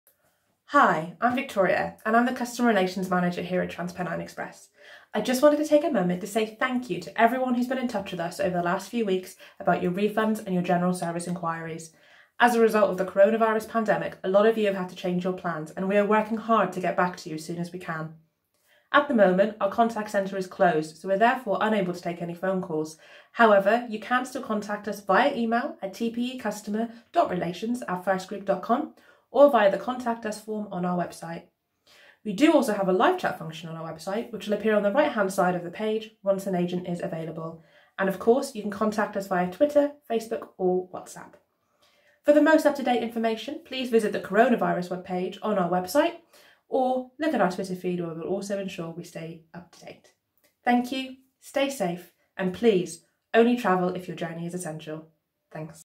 Victoria - Customer Relations Manager
Victoria has an update for customers regarding Coronavirus, our contact centre and the current situation with refund claims.